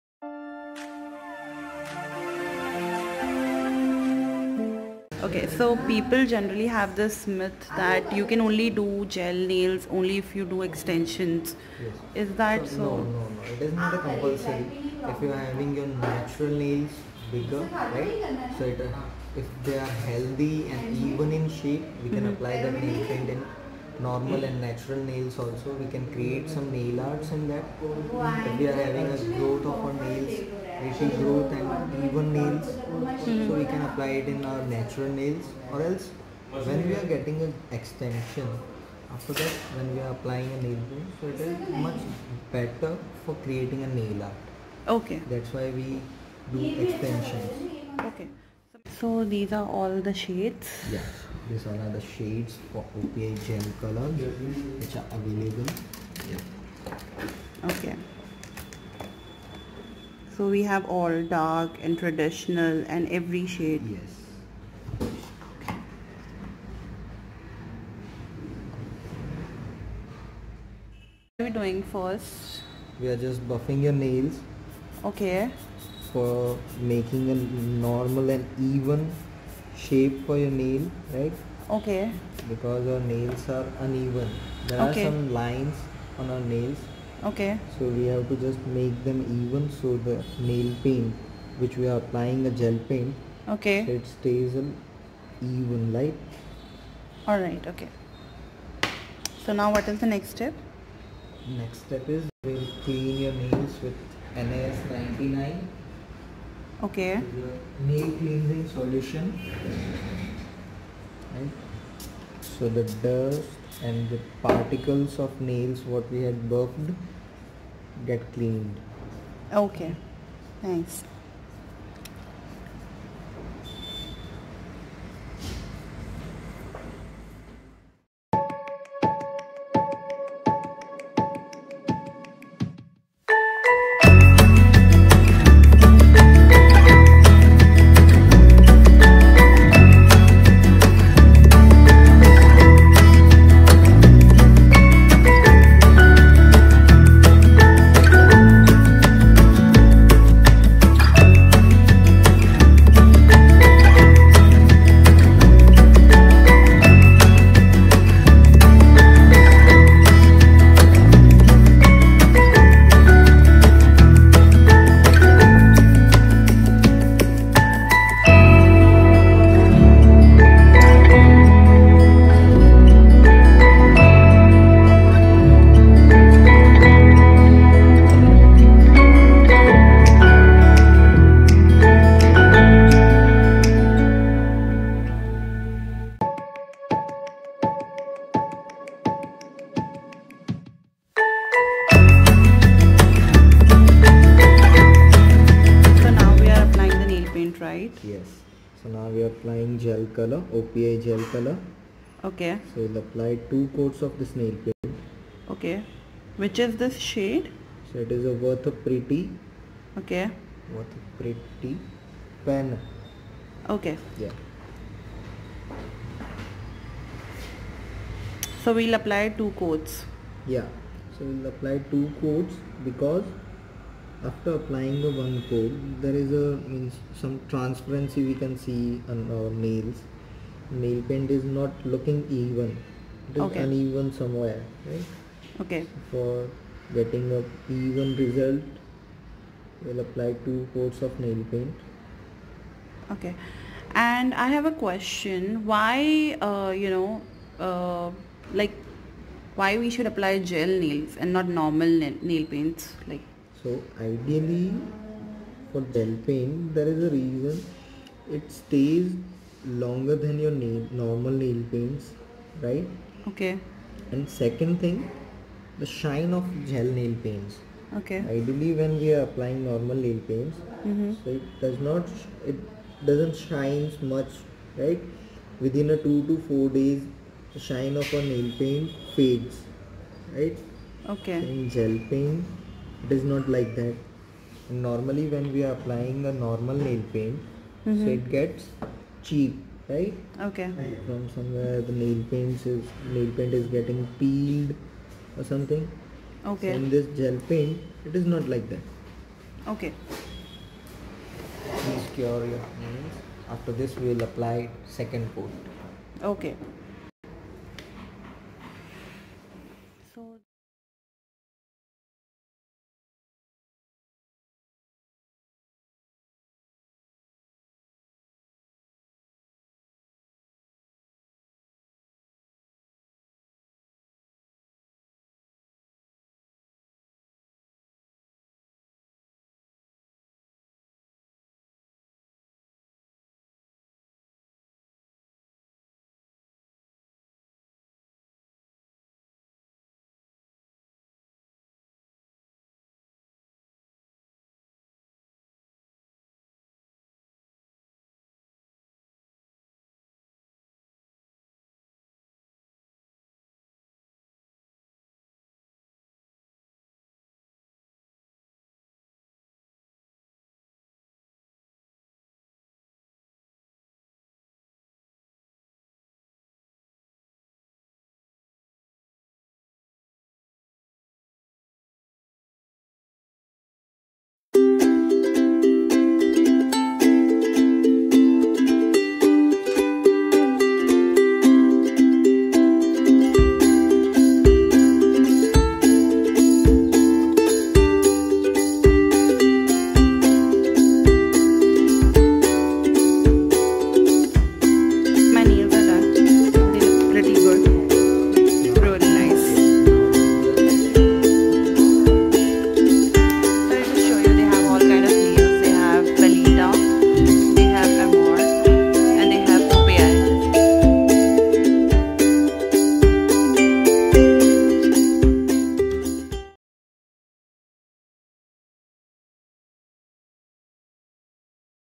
How to apply Gel nails paint | Gel nail paint step by step | BEAUTY TIPS
Hey everyone

I am rinku and i have shared my experience of getting gel nail paint done at enrich salon ahmedabad

This is my first vlog kinda video.. sorry for too muc background noise..

Hope all of u like this video